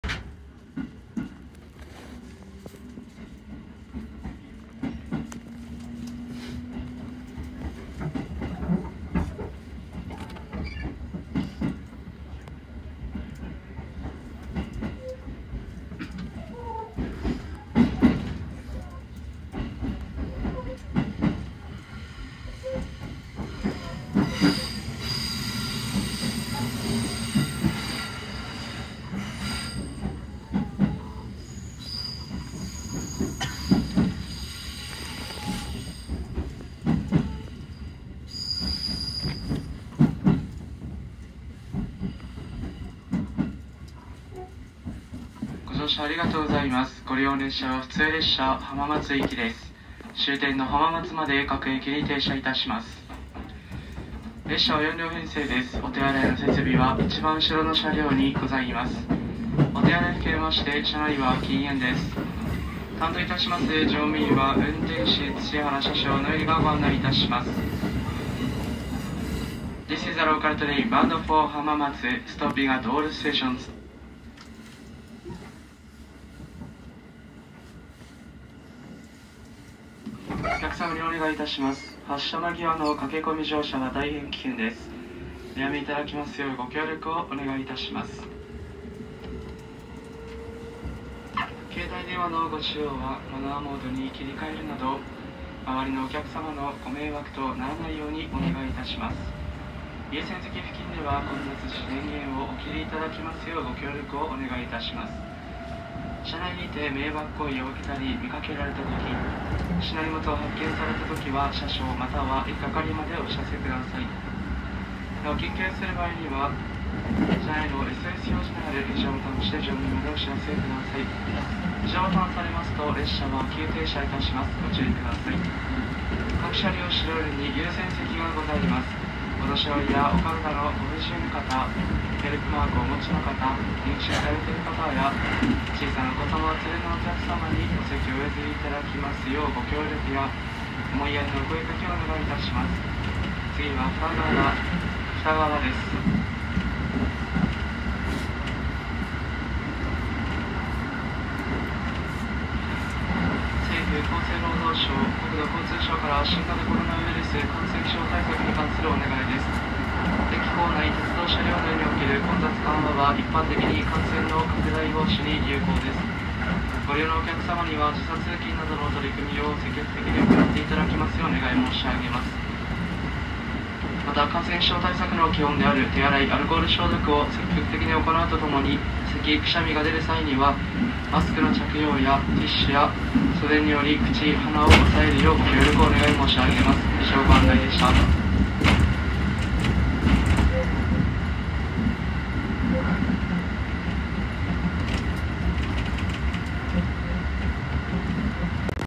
JR東海 新型コロナウイルスに関する放送
JR東海の 新型コロナウイルス に関するお知らせです。この日は用事でお出かけしていましたが、このままだと趣味でのお出かけはなかなかできないので、はやく収束して欲しいですね。

新型コロナウイルスのお知らせは 2:34 あたりから流れます

please thumbs up and sub‼︎